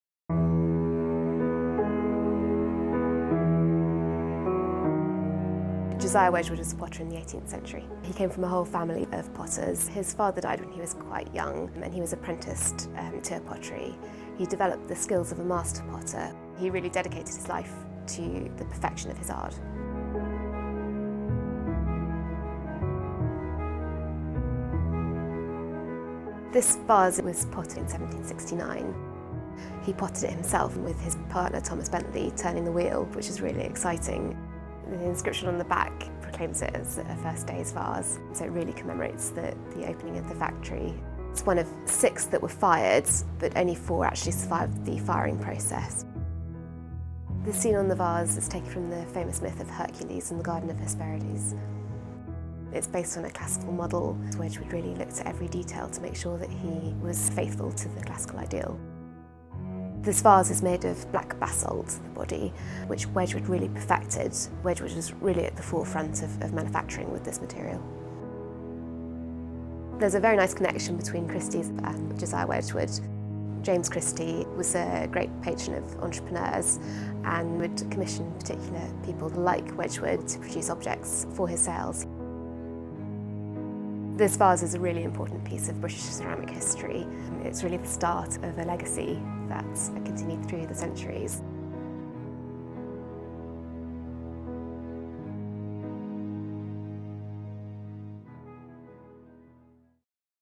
A Wedgwood Vase from 1769 – 'An Important Piece of Ceramic History'
‘This vase is a really important piece of British ceramic history,’ says Matilda Burn, a Christie’s specialist in Decorative Arts, of this black ‘basaltes’ encaustic-decorated ‘First Day’s Vase’, potted by Josiah Wedgwood in 1769. ‘It’s the start of a legacy that has continued through the centuries. He really dedicated his life to the perfection of his art.’

Josiah Wedgwood (1730-95) is arguably the most revered of British potters. Coming from a large family of well-established potters in Staffordshire, his upbringing was modest, although he was well connected to important figures in the pottery industry. These included his cousins Thomas and John Wedgwood of the Big House, who were successful manufacturers of salt-glazed stoneware.

Following the death of his father, Josiah Wedgwood was apprenticed to his older brother, Thomas. ‘He went on to develop the skills of a master potter,’ explains Burn, ‘which was really the most coveted position and which took years to work his way up to.’

Wedgwood established his own manufactory, the Ivy House Works, with his cousin Thomas in around 1759. Building on early successes, he moved shortly after this to the Brick House Works. During this time he continually carried out experiments to finesse his creamware body and lead glaze. It was with his creamware body that Wedgwood achieved wide acclaim and commercial success.

Josiah was a great publicist of his own wares and during the 1760s he had a growing list of aristocratic patrons, all of whom sought his fashionable creamware. Wedgwood also keenly courted the patronage of Queen Charlotte, who commissioned a service from the Chelsea factory in 1762.

It was on a visit to Liverpool in 1762 that Wedgwood had a chance meeting with Thomas Bentley, a well-travelled and cultivated man who had taken the Grand Tour in 1753. As a business partner Bentley offered Wedgwood not only essential commercial experience but also a deep understanding of changing tastes and market trends. In combination with Wedgwood’s inventiveness and deep technical understanding of the art of pottery, Bentley was able to help shape and guide the direction of production.

Together, Wedgwood and Bentley played an important part in the development and expansion of the Trent and Mersey Canal. The success of the canal scheme allowed Wedgwood to expand his manufactory, and he purchased a 350-acre estate through which the canal would pass. The new purpose-built factory and the surrounding estate became known as Etruria, named after the ancient central state in Italy whose arts, most notably pottery, were being rediscovered in archaeological digs at the time.

This ‘First Day’s Vase’ was potted on the opening day of the Etruria Factory in Staffordshire on 13 June 1769. Wedgwood potted it himself, with his partner Bentley turning the wheel. ‘It is one of six that were fired,’ explains Burn, ‘but only four survived the firing process, which it makes it all the more exciting.’